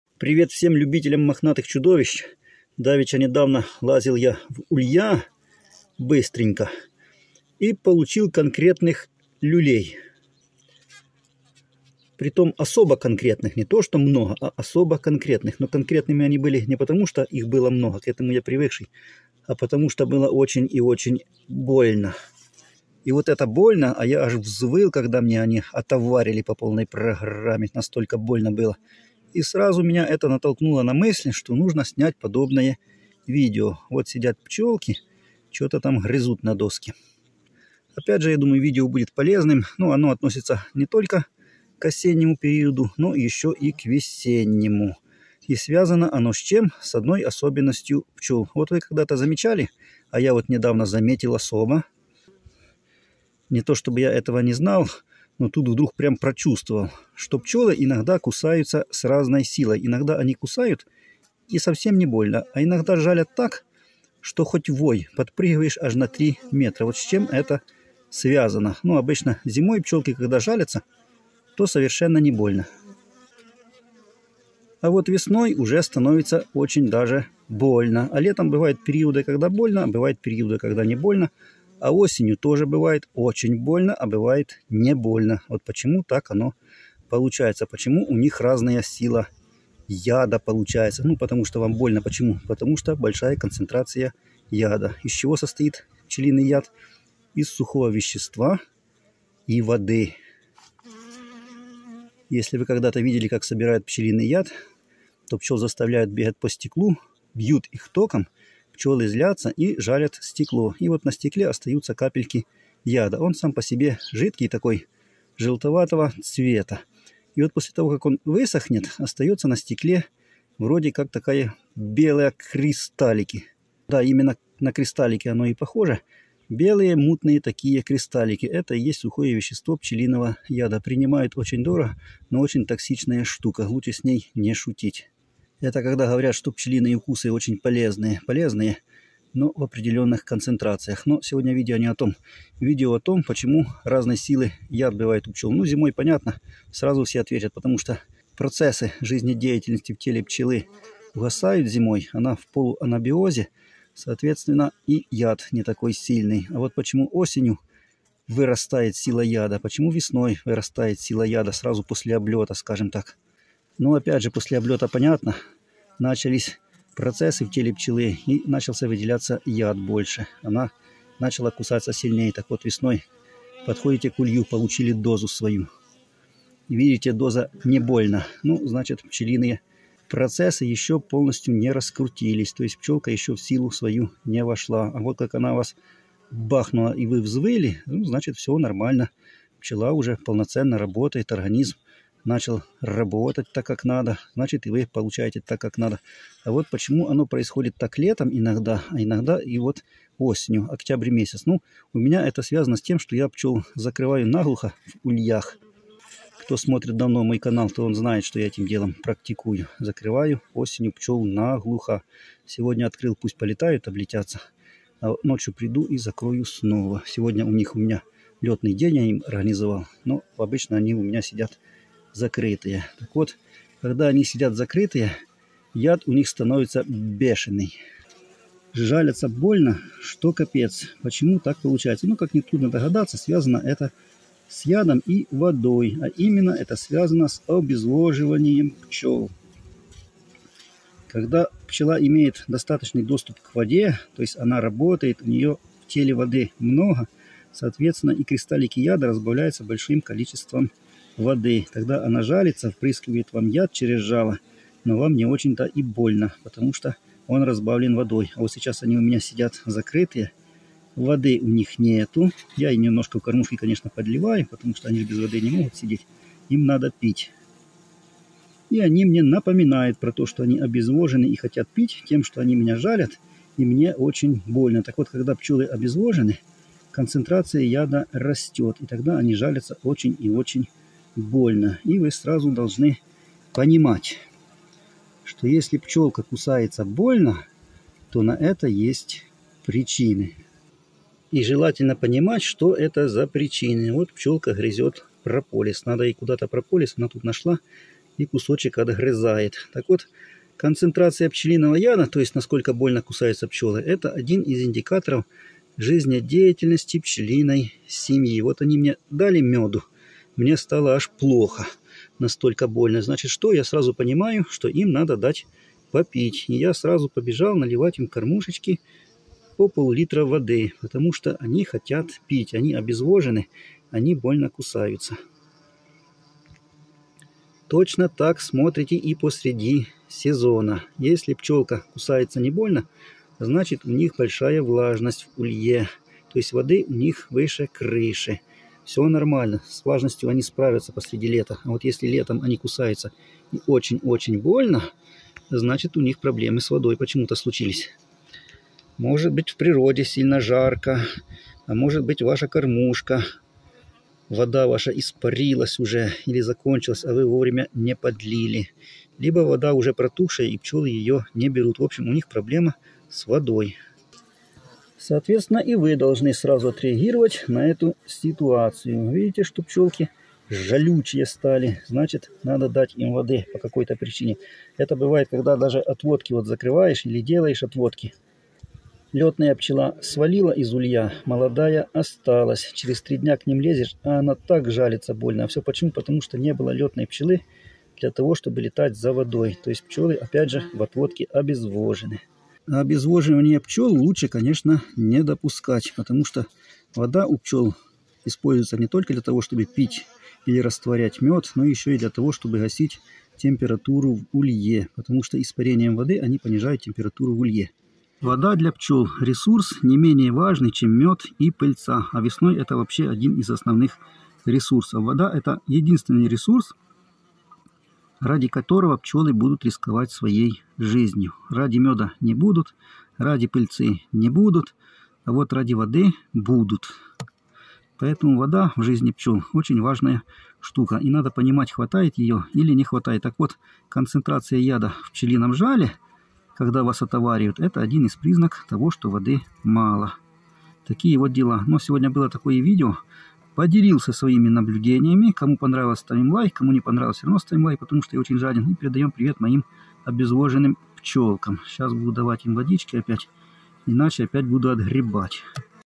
Пчёлы. Бывает период, когда на пасеке пчёлы очень больно жалят. Что это значит и как исправить.
Если пчёлы в отводке, или в семье начали очень больно жалить, на это есть одна серьёзная причина. Которую нужно очень быстро исправить